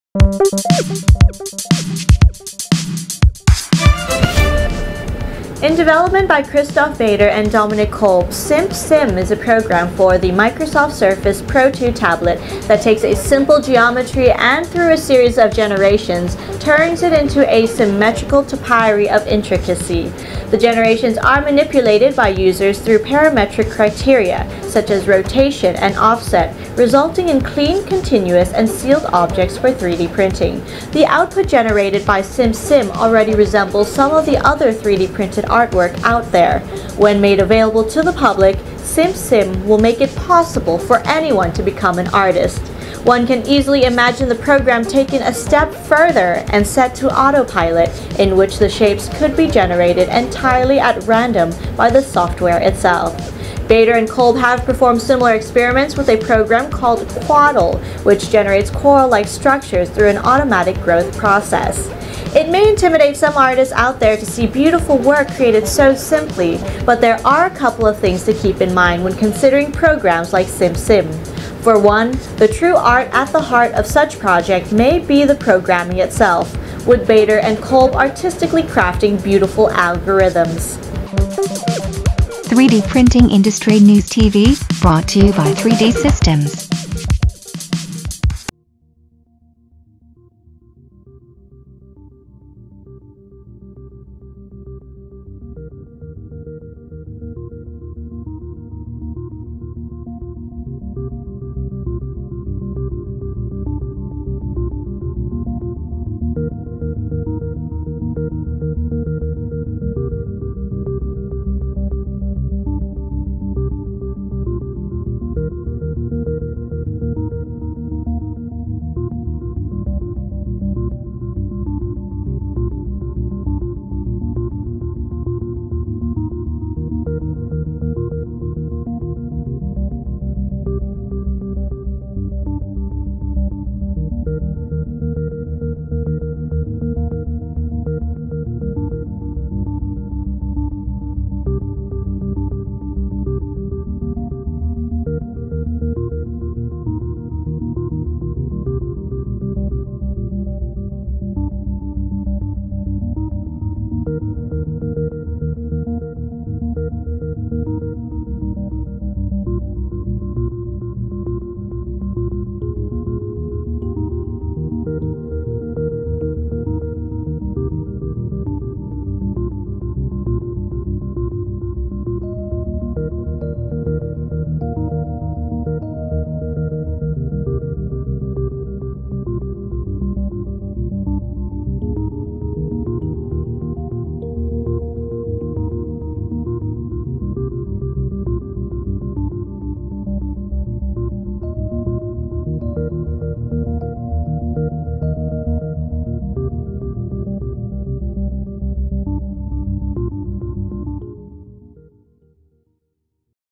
Generating complex shapes for 3D printing
Video transcript: In development by Christoph Bader and Dominik Kolb, SimpSymm is a program for the Microsoft Surface Pro 2 tablet that takes a simple geometry and, through a series of "generations", turns it into a symmetrical topiary of intricacy.  The generations are manipulated by users through parametric criteria, such as rotation and offset; resulting in clean, continuous and sealed objects for 3D printing.

The output generated by SimpSymm already resembles some of the other 3D printed artwork out there.  When made available to the public, SimpSymm will make it possible for anyone to become an artist.  One can easily imagine the program taken a step further and set to "autopilot", in which the shapes could be generated entirely at random by the software itself.  Bader and Kolb have performed similar experiments with a program called QUADDEL, which generates coral-like structures through an automatic growth process.

It may intimidate some artists out there to see beautiful work created so simply, but there are a couple of things to keep in mind when considering programs like SimpSymm.  For one, the true art at the heart of such projects may be the programming itself, with Bader and Kolb artistically crafting beautiful algorithms.